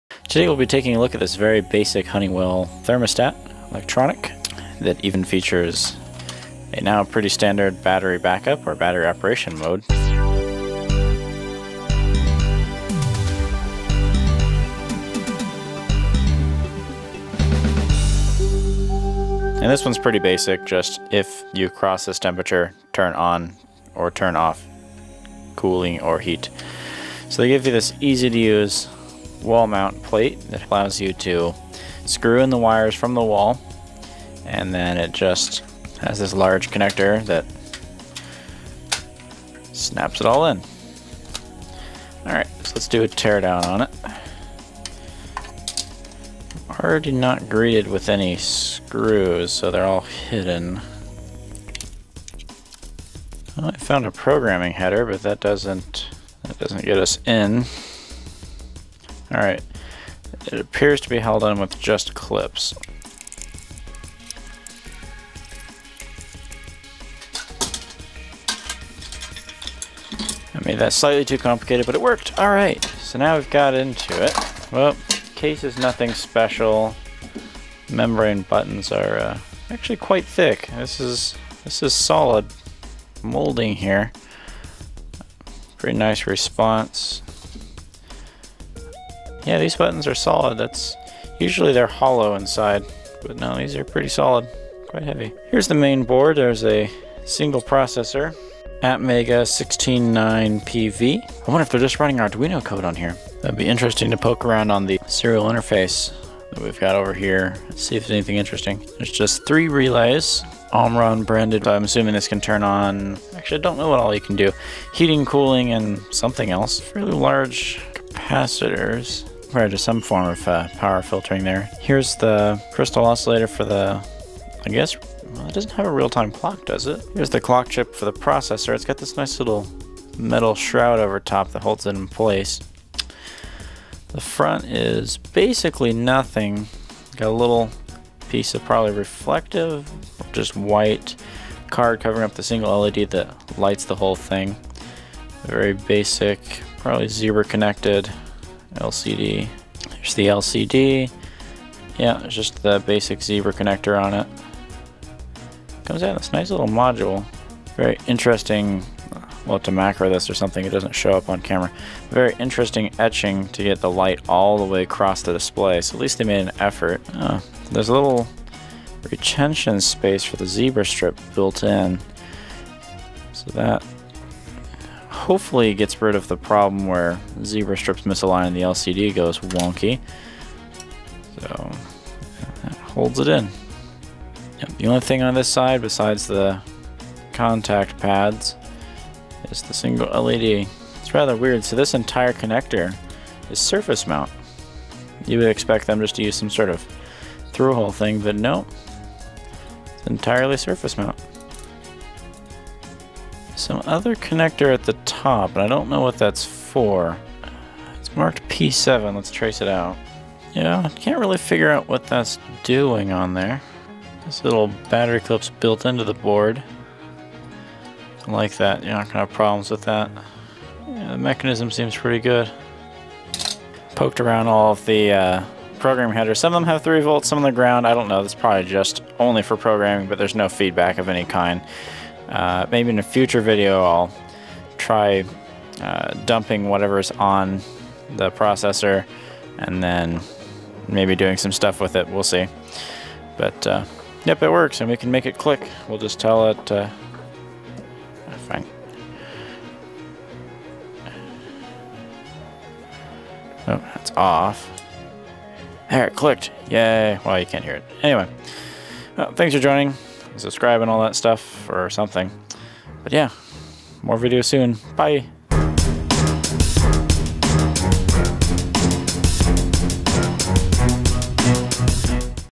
Honeywell Basic Electronic Thermostat Teardown
This is a very basic thermostat unit with a nice internal layout.